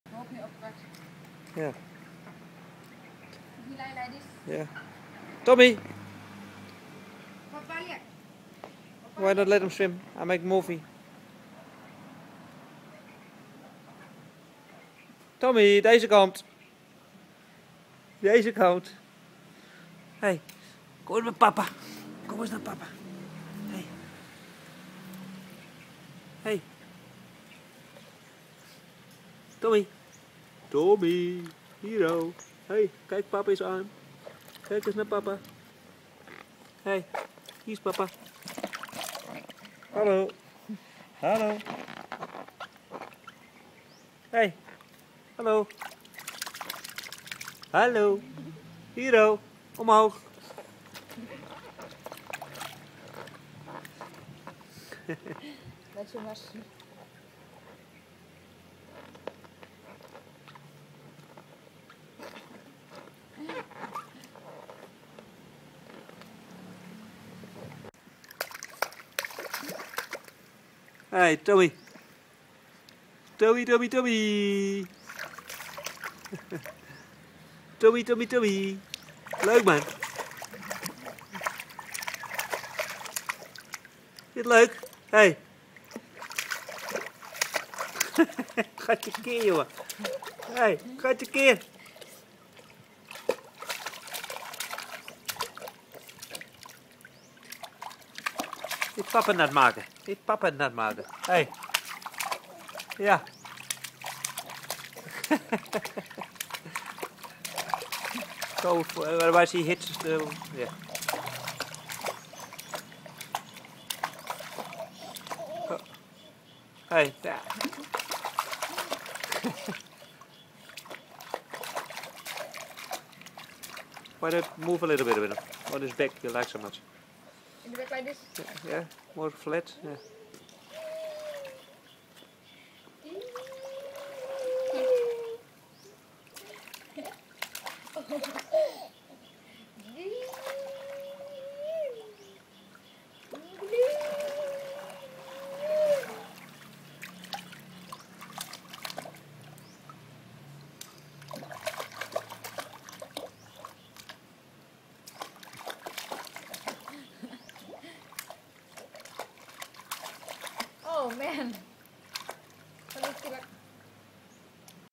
Tommy in swimming band
Mama is in the pool with Tommy. Tommy is in a swimming band and enjoys it a lot.